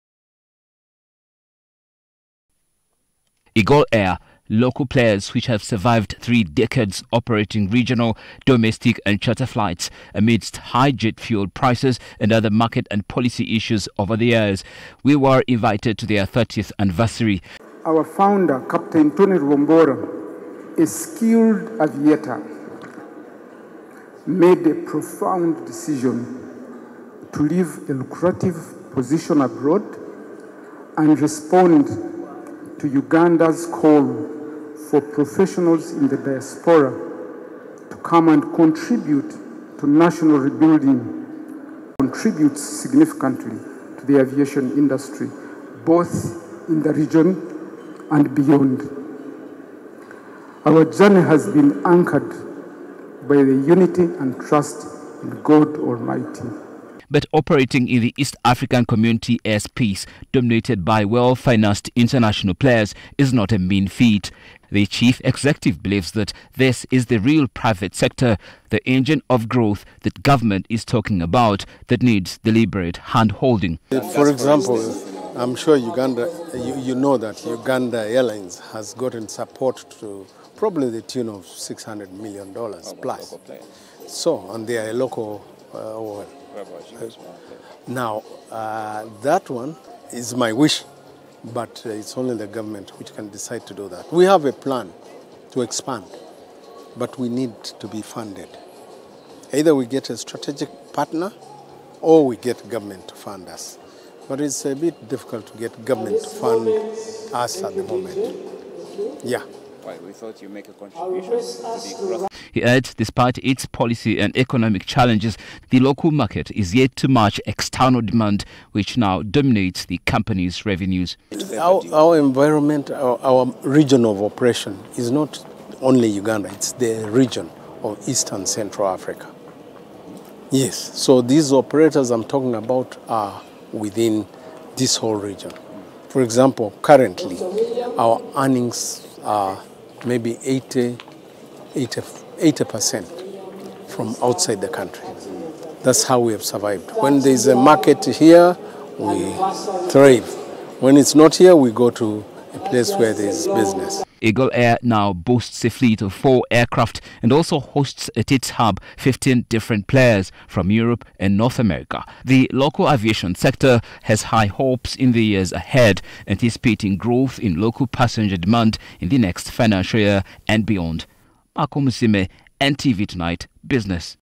Aviation sector local players too deserve government support
Local aviation players operating domestically and regionally are hopeful that the government will extend financial support to the sector similar to the support given to Uganda Airlines.

Tony Rubombora, the founder of Eagle Air, expressed this sentiment during the airline's 30th-anniversary celebration. He highlighted that providing support to private sector players in this capital-intensive and turbulent industry would significantly contribute to the country's economic growth. Experts view 2024 as a recovery year for the aviation sector, as African airlines have surpassed pre-Covid levels in capacity and traffic.